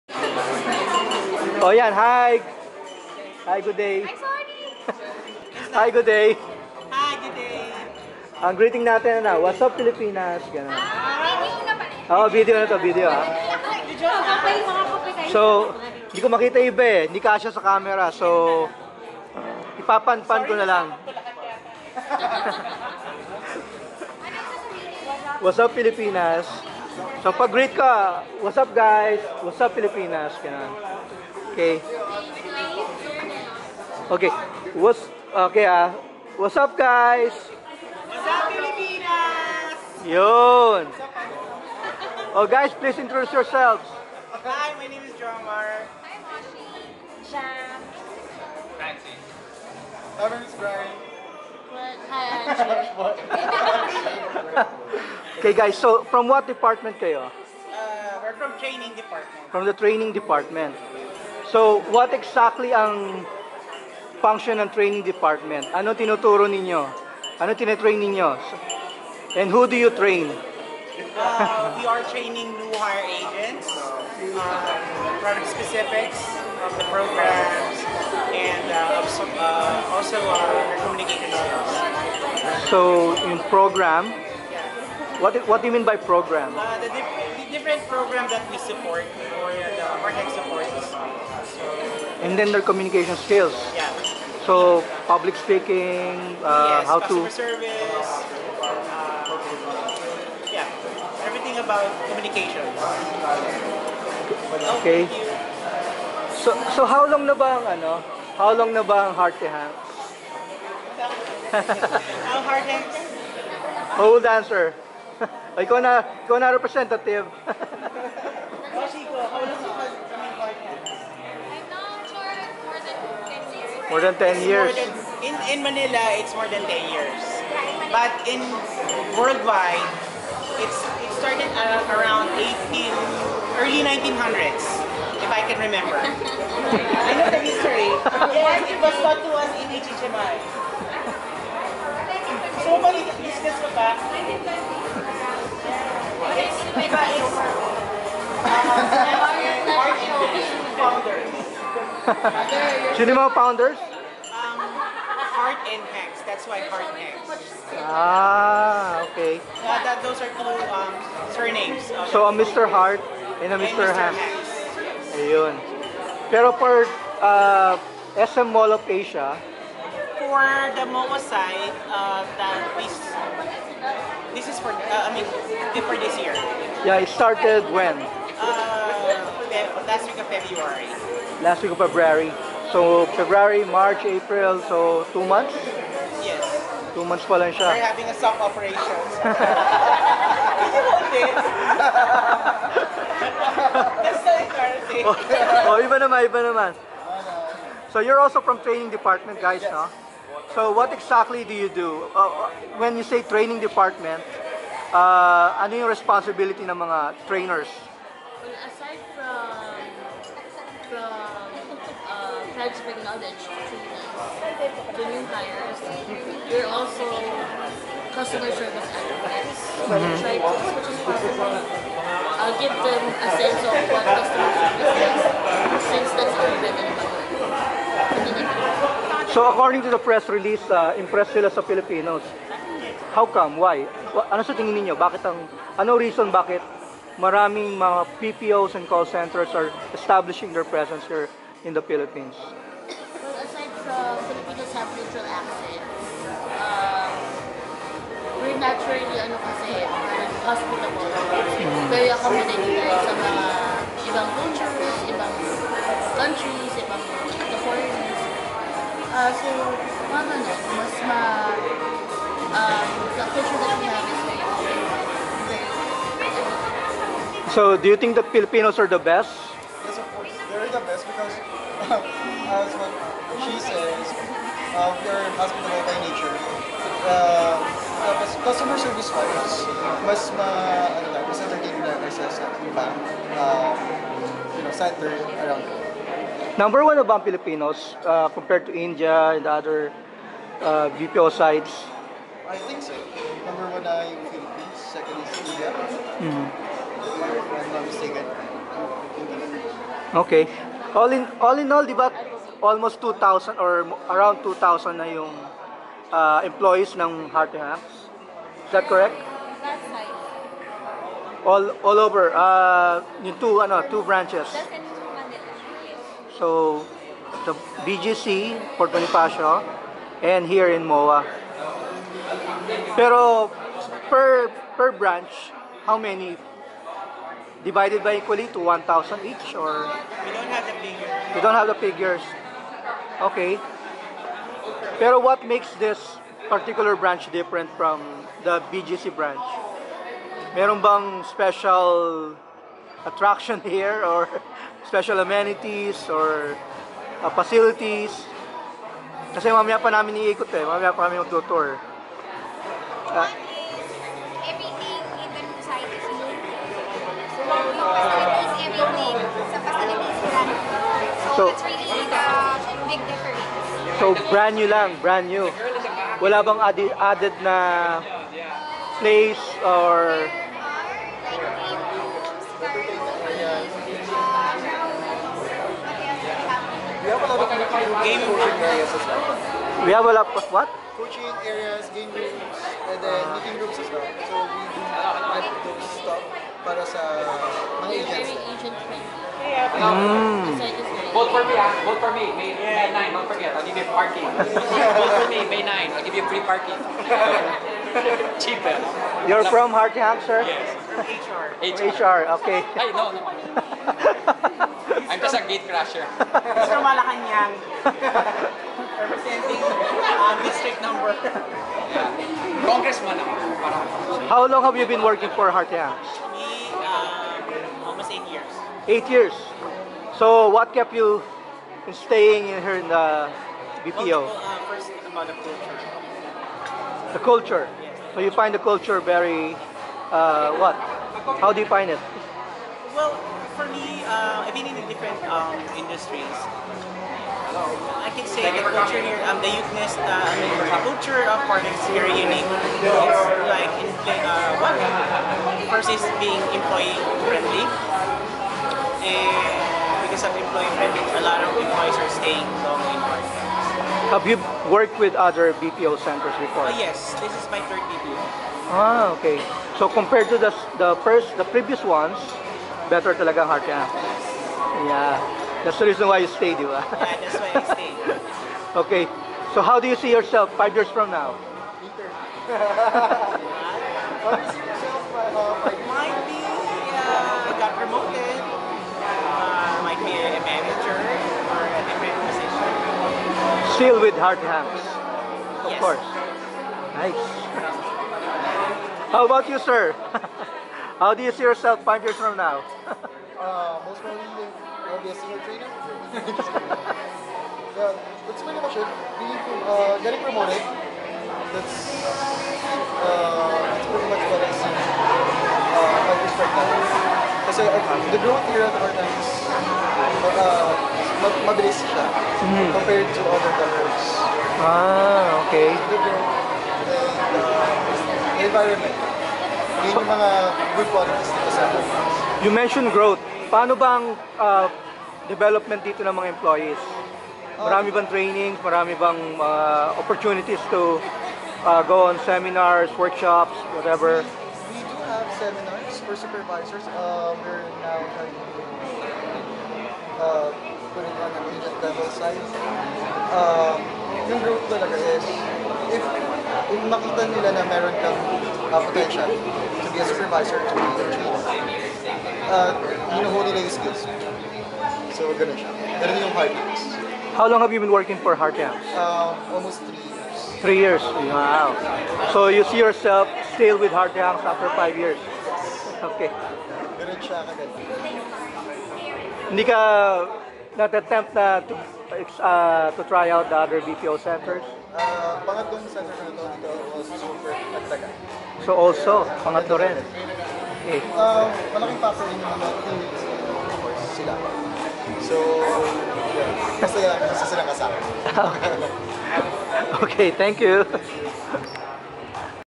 Harte Hanks MOA Launch Interview
Interview with some of the Training Department employees of Harte Hanks while waiting for the program to start. It's called spending your time wisely and productively to get to know more insights about the event and the company. and of course, its people.